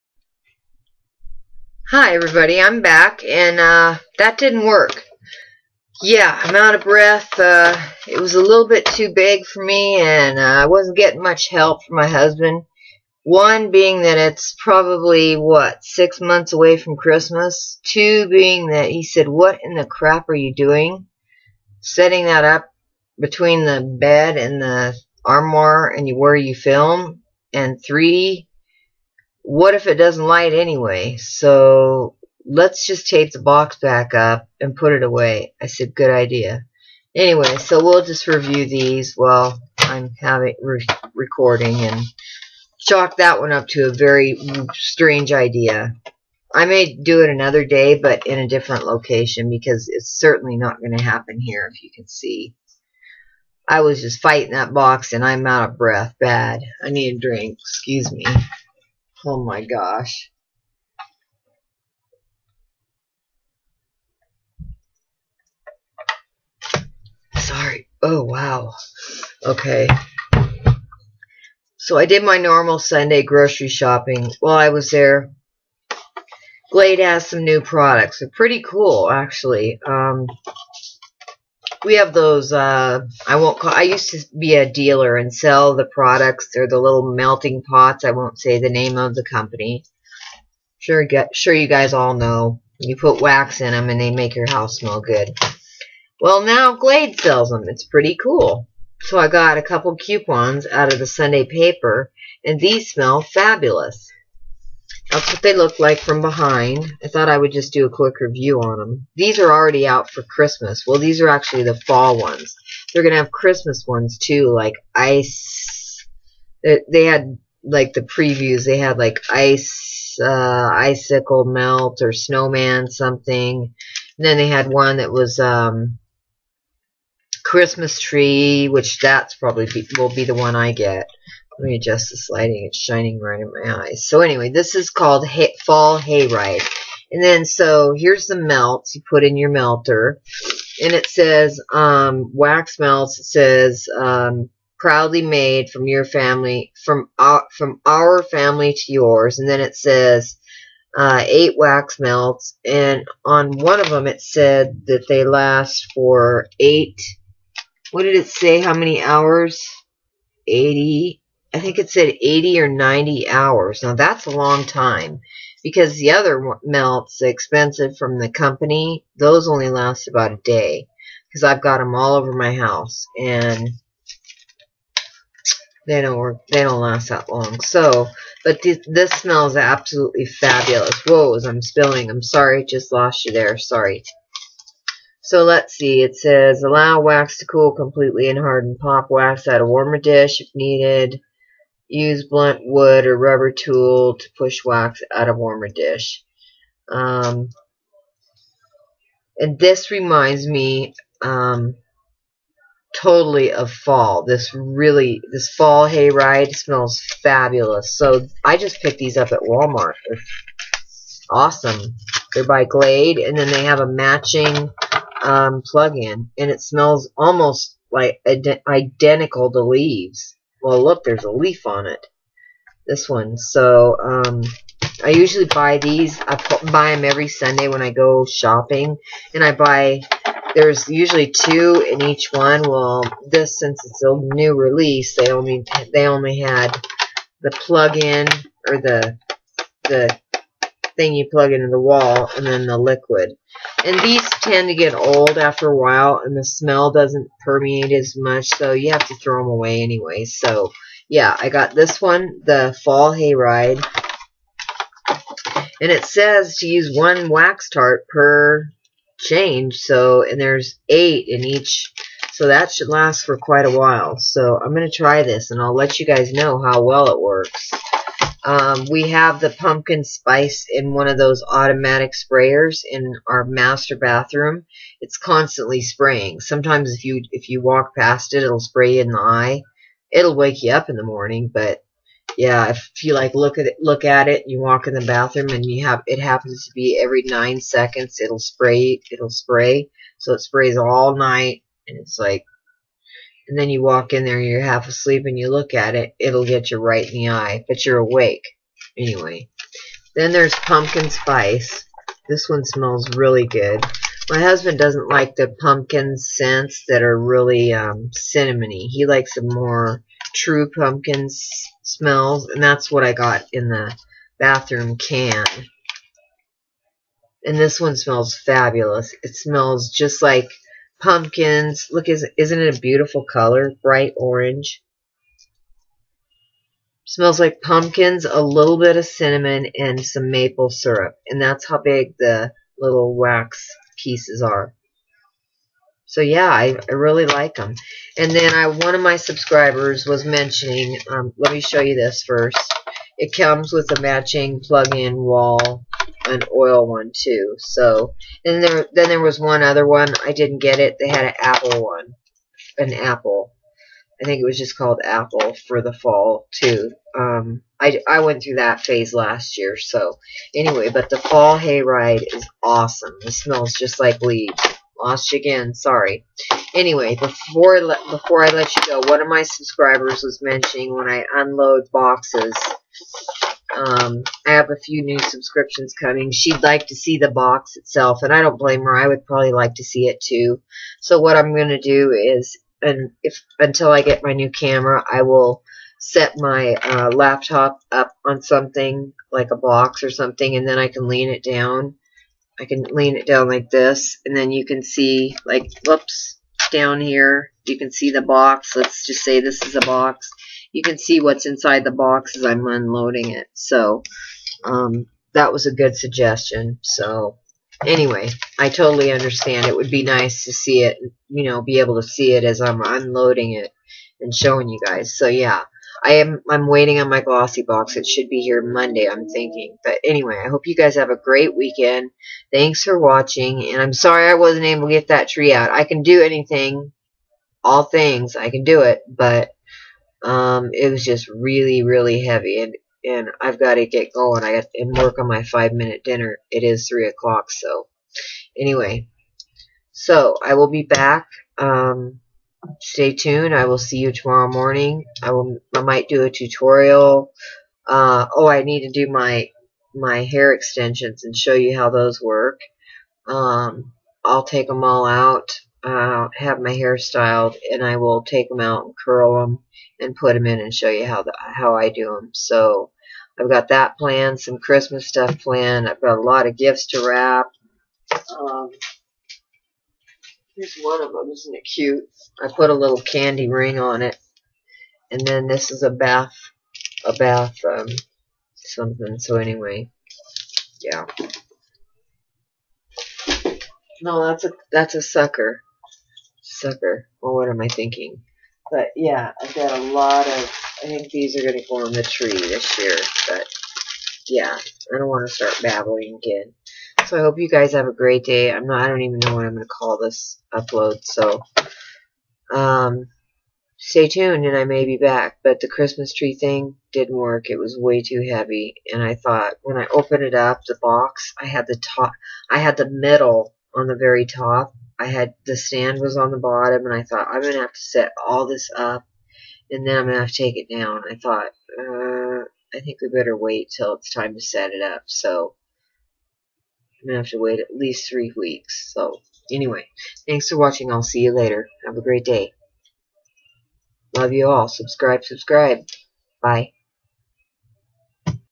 Bad Idea xmas tree,Scent review......
Well I gave it a good try,1.The artificial tree(flocked)way too heavy,2.Way too early,shall I go on. So I will open the tree box and make sure it works but even if it didn't can't exchange it until it gets a little closer to xmas. So instead Idid my Sunday Grocery shopping and picked up some new Fall Scent products and reviewed those, any questions inbox me.